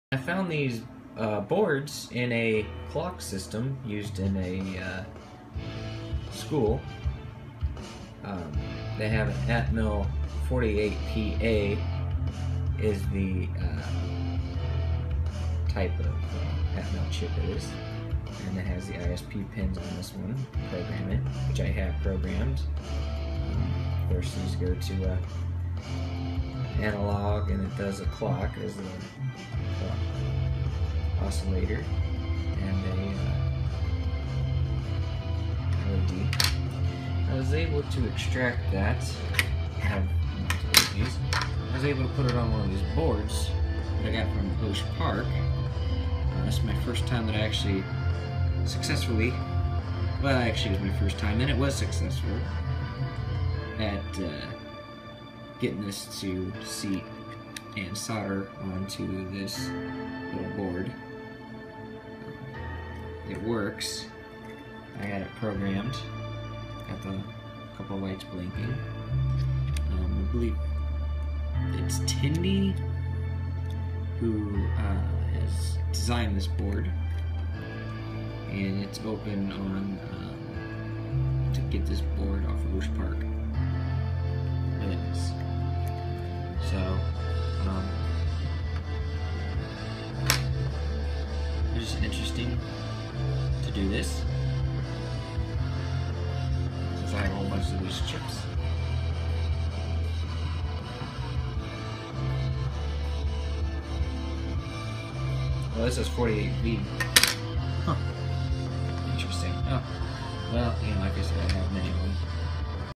RedBit One w/ Atmel ATMega48pa
Found some old Atmel microprocessors, decided to make them useful by putting them on a development board I found on OSHpark designed by Tindie.

This was my first attempt at soldering an SMD.  It seems to be working!